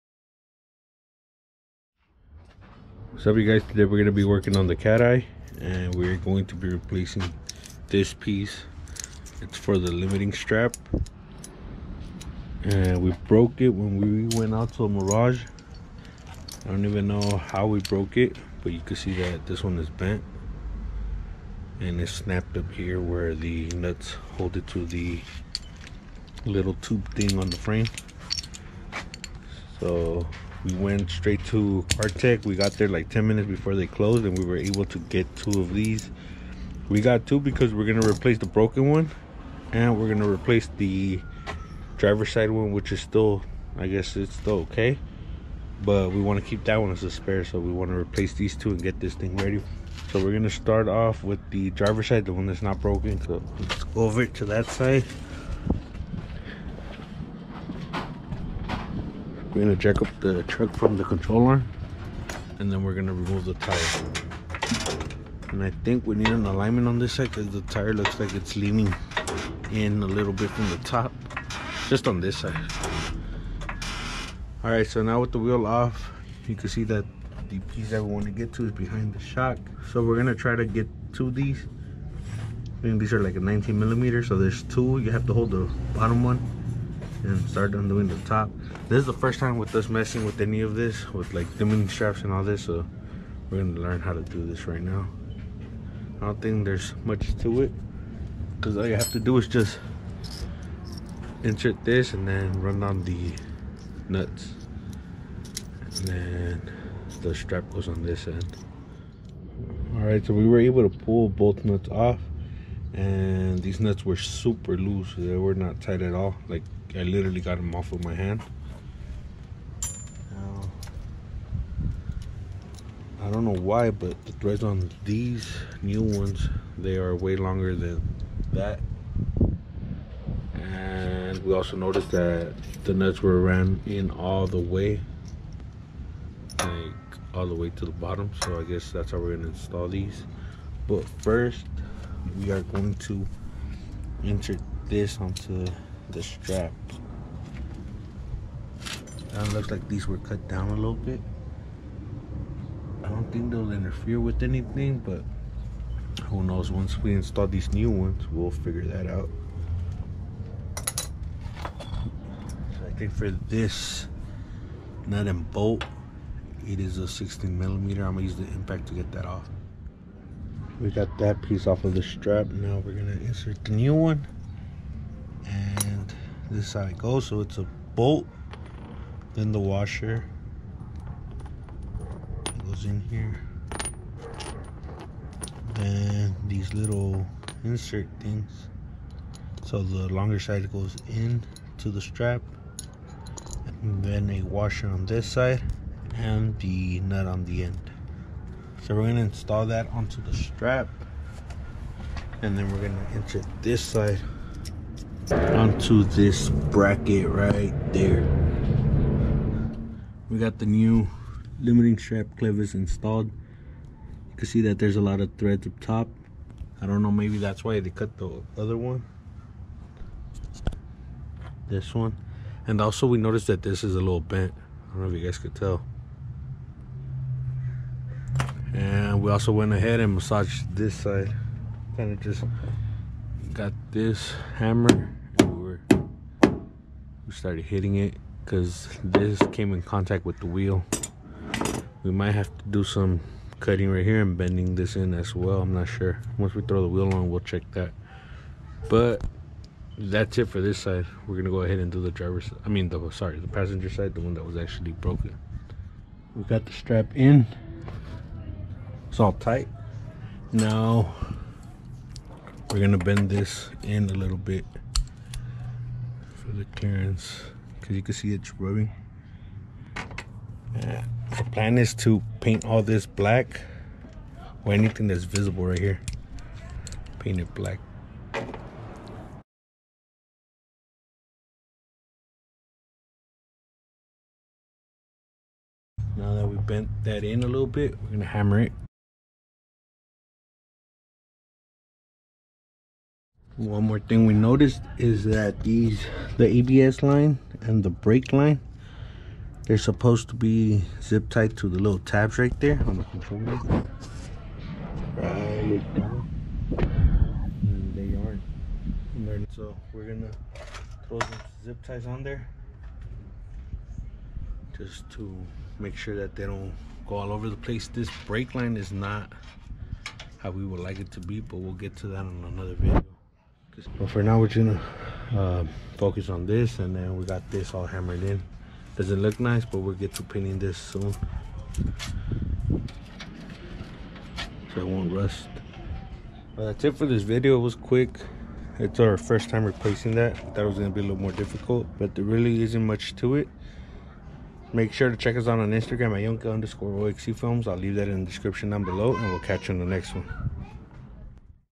Replacing The Limiting Strap Clevis On The Chevy Pickup
We broke a strap clevis on our trip to El Mirage so now we have to replaced it to get it ready for our trip to Glamis.

Yonke_Oxcfilms